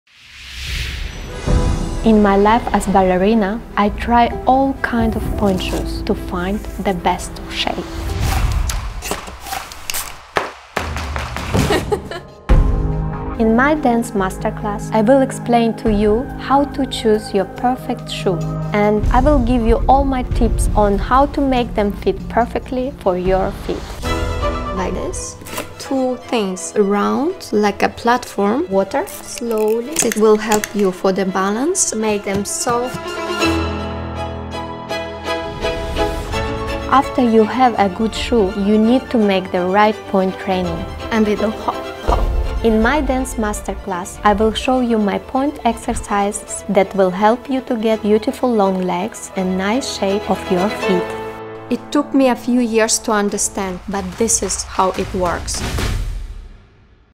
BALLET POINTE SHOES by Star Ballerina Iana Salenko | Dance Masterclass
BALLET POINTE SHOES Dance Masterclass from Iana Salenko!
Iana explains everything you need to know about pointe shoes and how to achieve beautiful lines in your pointe work.
If you are looking to discover and customize your perfect pointe shoes, as well as get inside tips from one of the top ballerinas in the world, this is for you.

Globally renowned for her grace and impeccable pointe work, Iana is a principal dancer with Staatsballett Berlin and guest artist at the Royal Opera House.